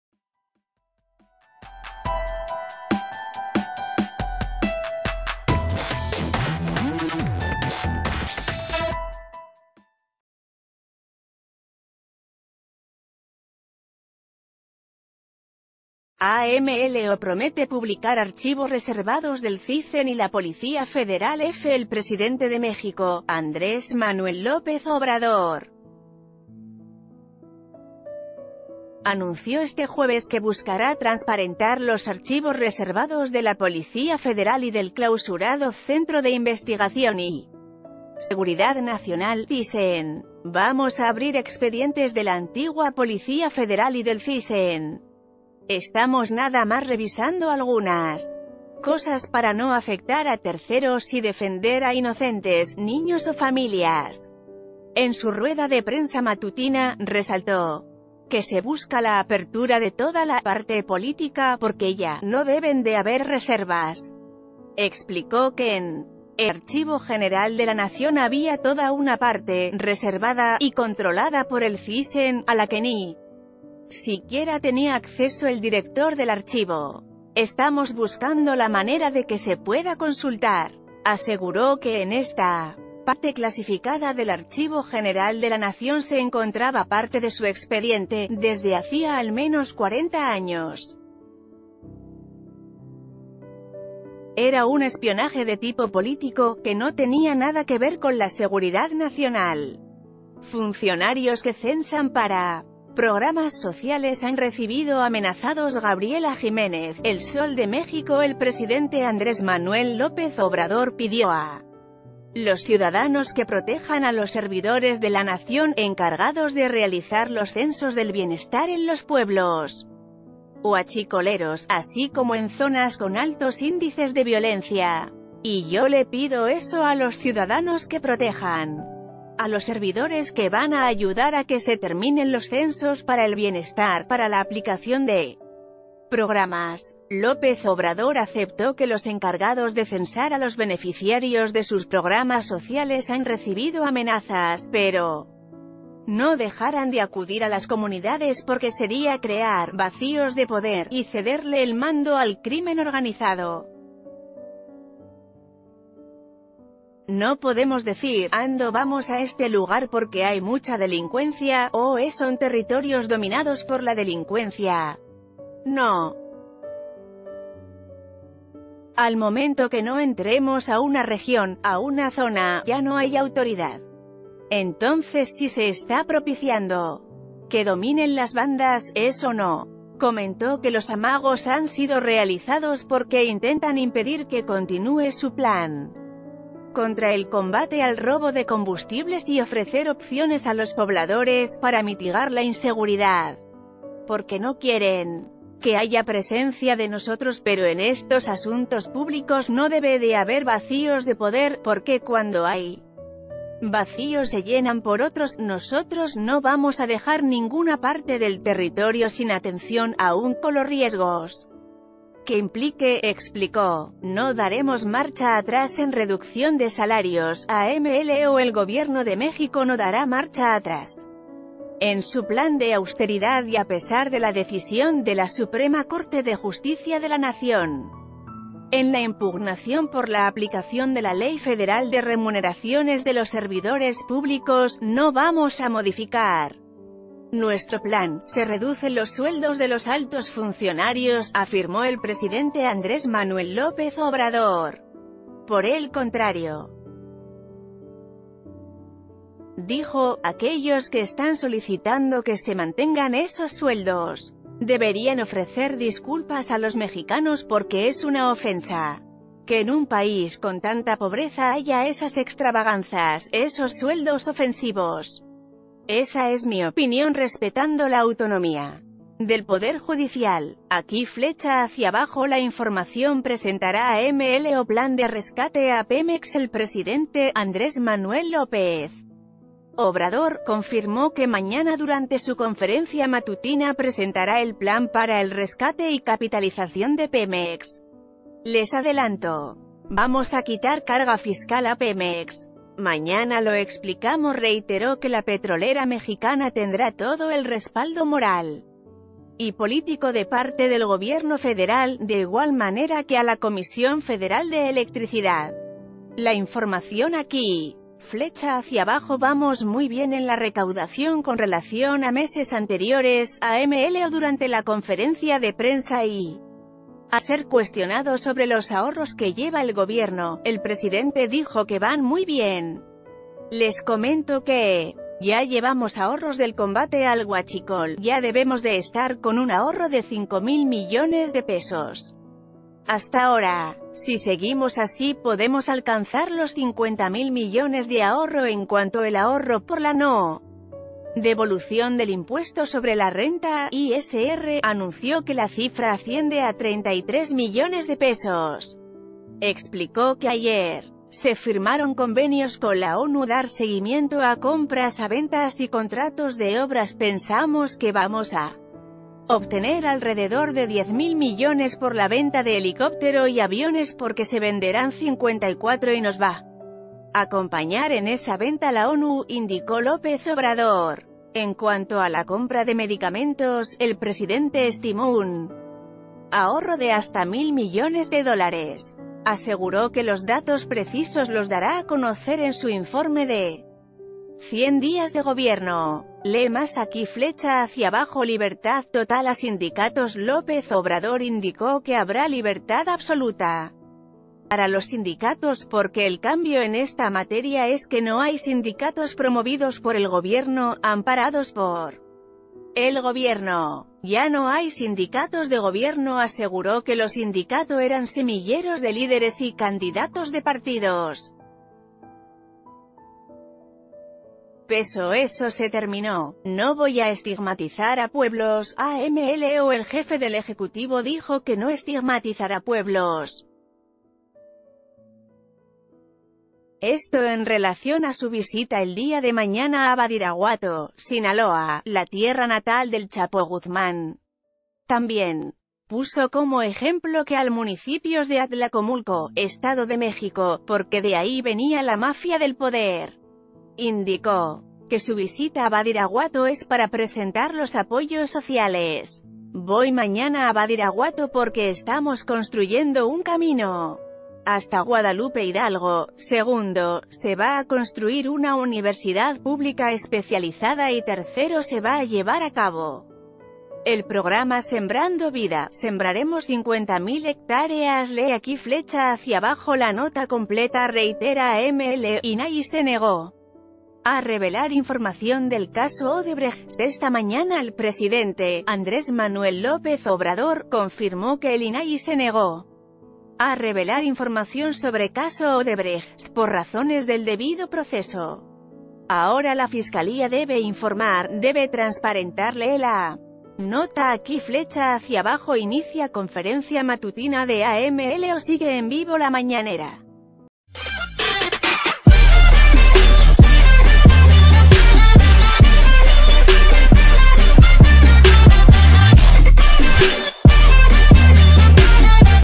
Aquí la agenda de AMLO este 14 de febrero
AMLO promete publicar archivos reservados del Cicen y la Policía Federal EFE El presidente de México, Andrés Manuel López Obrador, anunció este jueves que buscará transparentar los archivos reservados de la Policía Federal y del clausurado Centro de Investigación y Seguridad Nacional (Cisen). Vamos ...